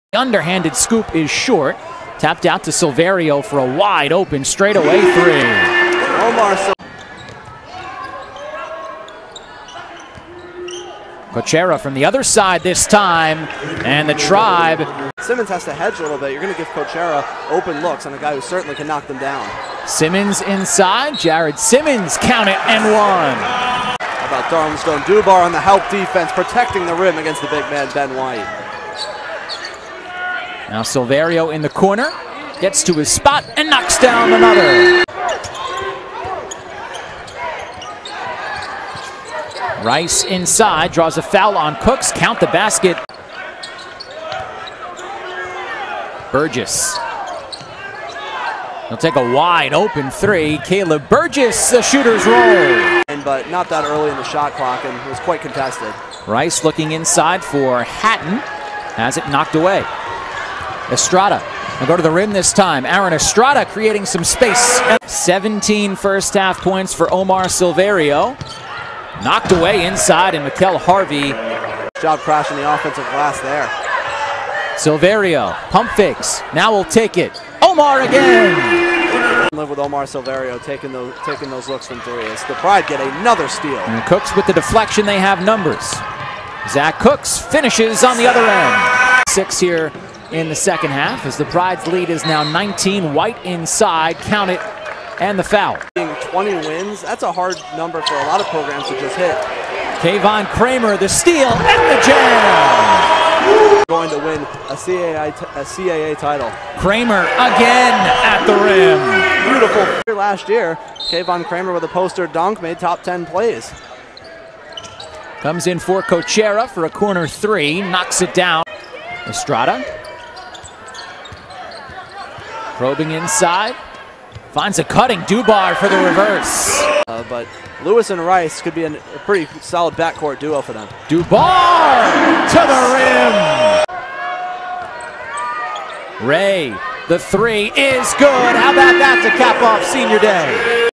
Men's CAAHoops Highlights: Hofstra 83, William & Mary 67 (2/26/22)
Omar Silverio scored 20 points and Jalen Ray added 19 as Hofstra locked up the No. 3 seed in the CAA Championship. Hofstra led by 10 at the break and built its largest advantage of the contest of 22 three times in the second half, all following Kvonn Cramer dunks.